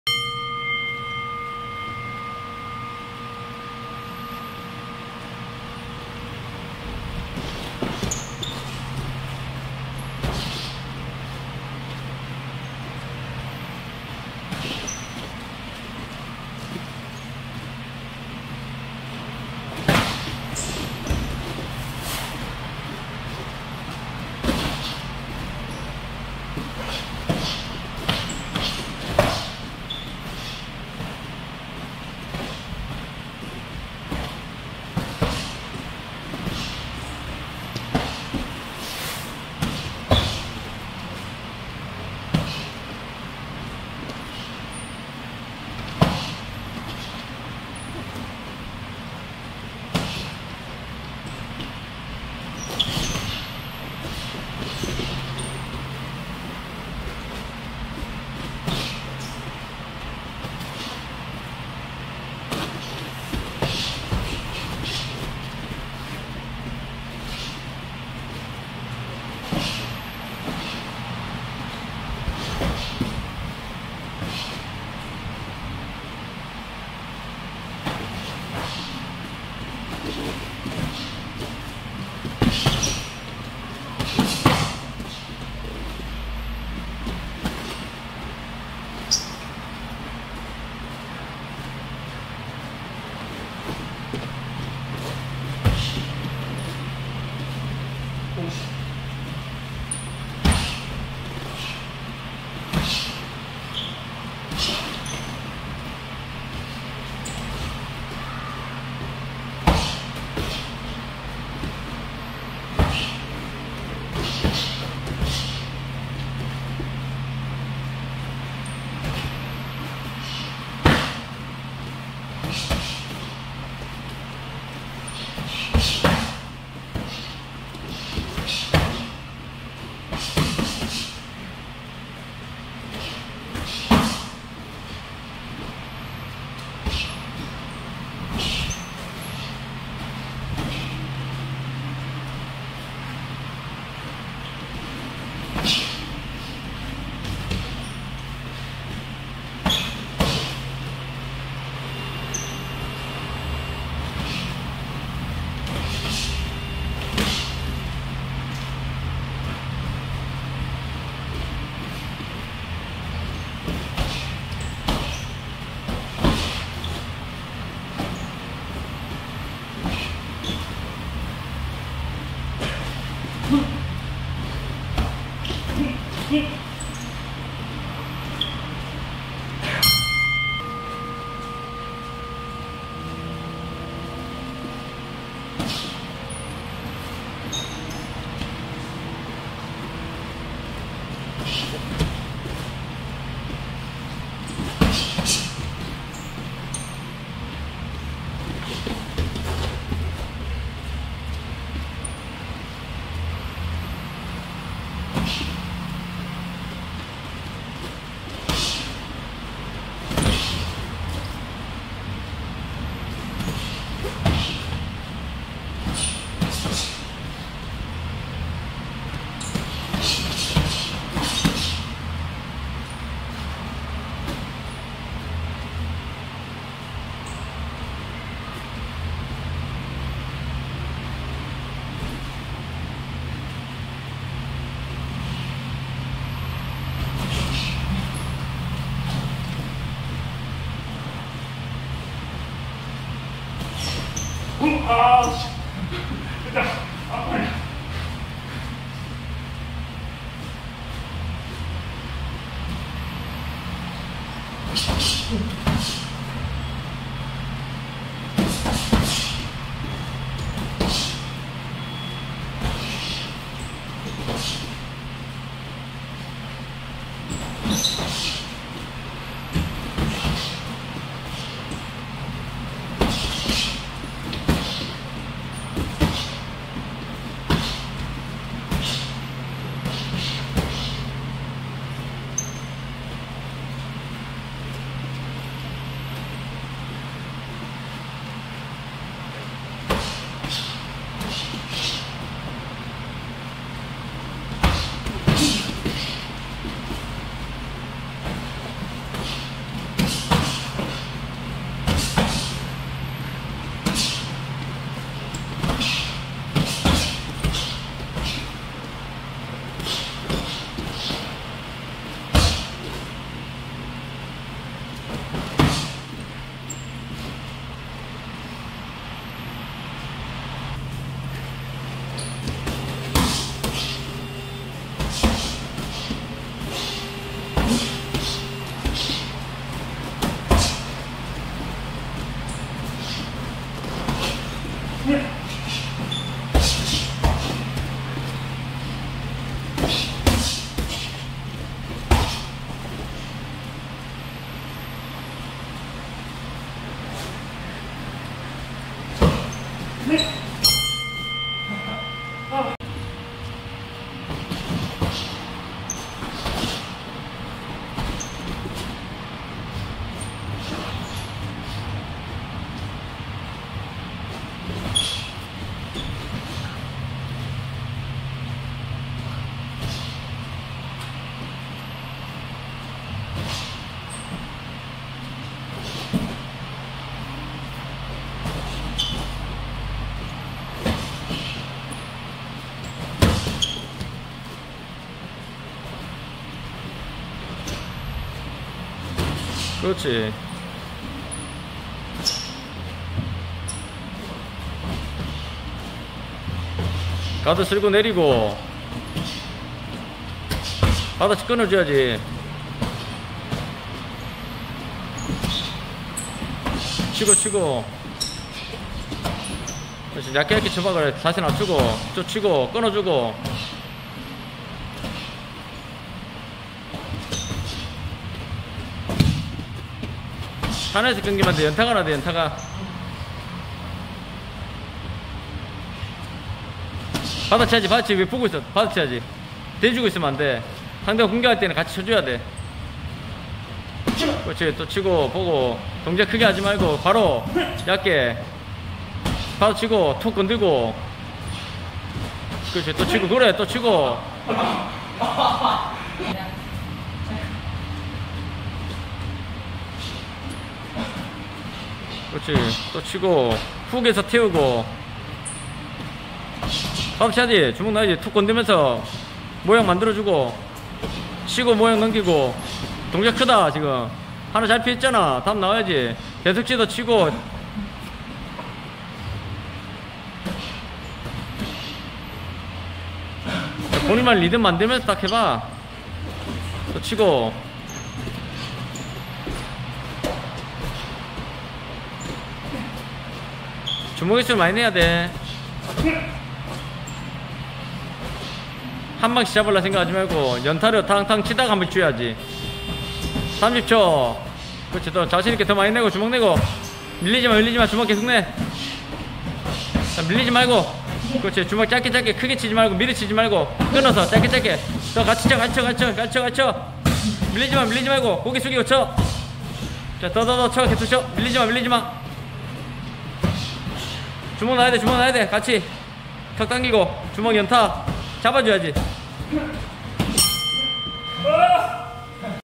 60kg 프로선수 VS 80kg 생체회원님 실전 스파링!! 2차전 달라졌을까!??
이은식복싱클럽(천곡점) 실전 스파링 훈련 영상입니다.